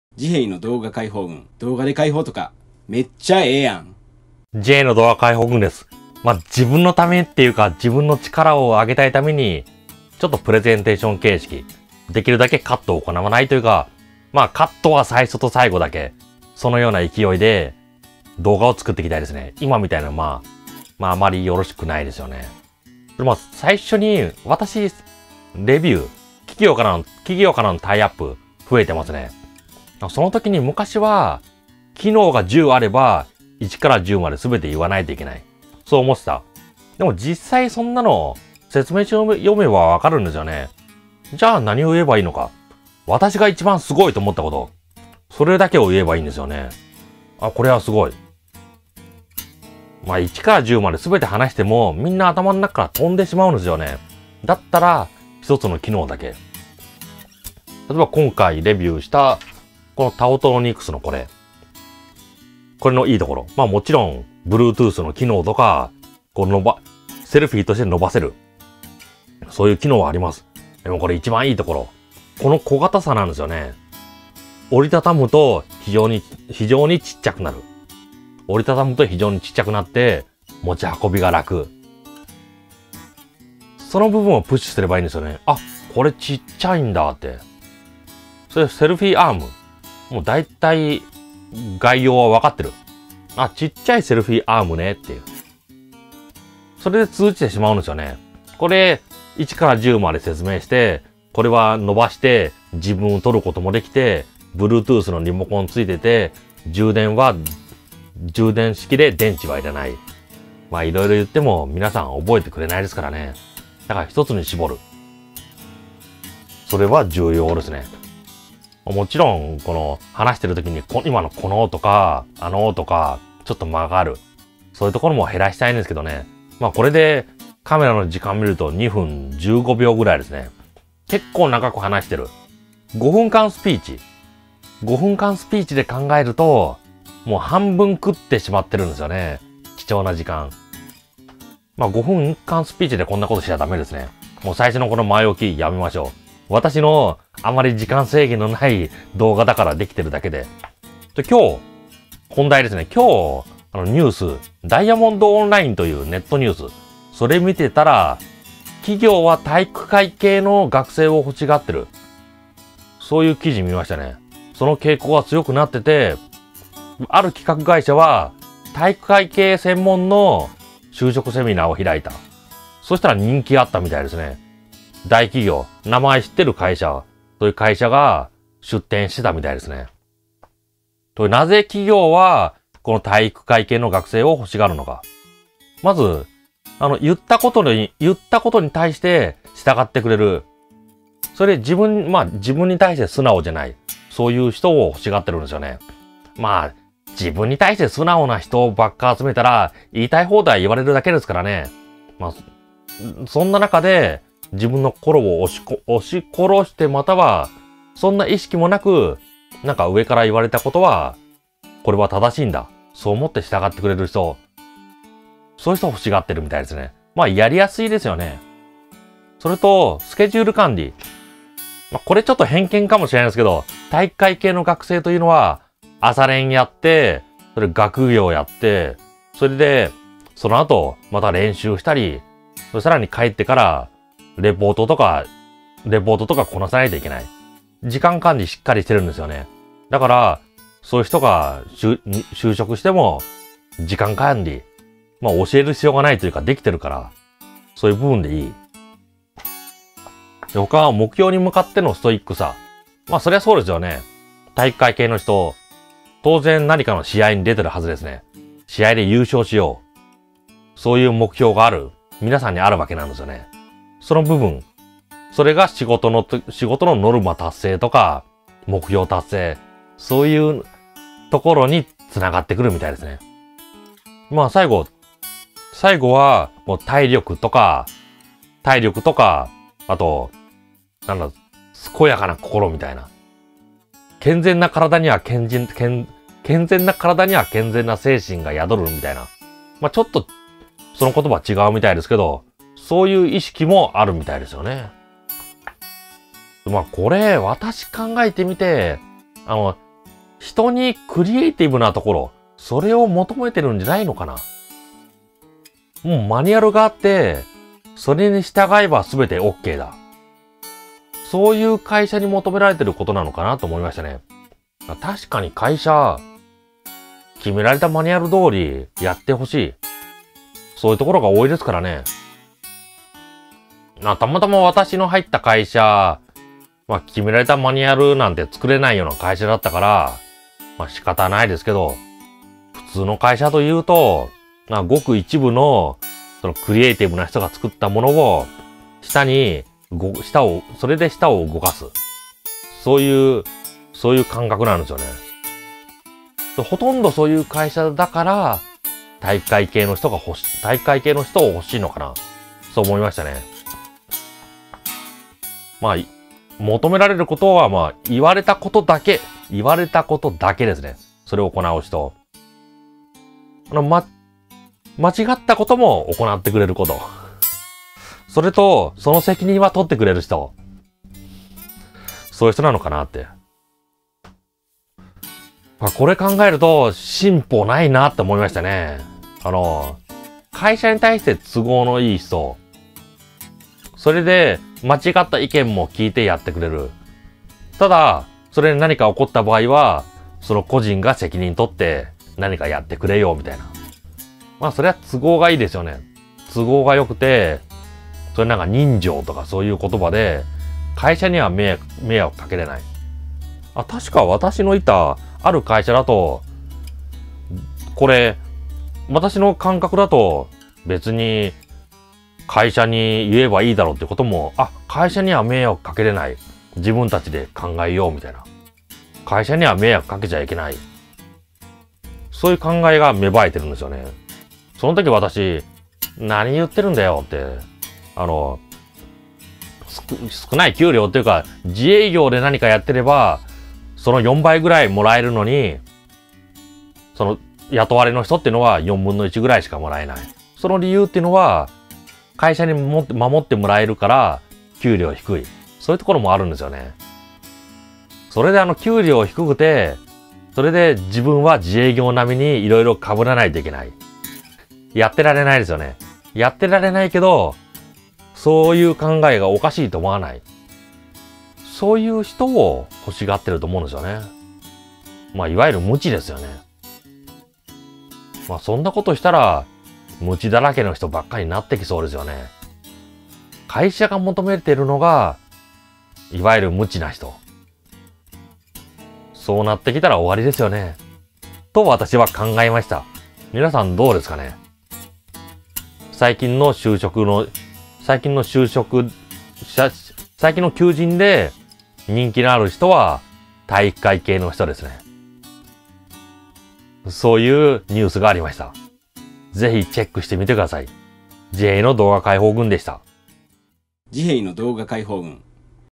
求められている人材は体育会系？ 盲目的に従ってくれる新人が欲しいらしい ストレートには言っていないが
動画解放軍LINEアカウントに登録してください。　便利な情報をお届けします。(スマホからクリックしてください)

体育会系学生が就活では強いと言われています。　大きな理由は、トップダウンの指示に何も疑問を持たずに従ってくれるところです。

確かに今までどおり会社のシステムとして動いていたシステムなので、それに盲目的に従うことは安全で効率的なのではと考えられます。　ただ、企業の活動って全てトップダウンで動く人だけで良いのだろうか？
ここが私のちょっとした疑問です。

ダイヤモンド・オンラインの記事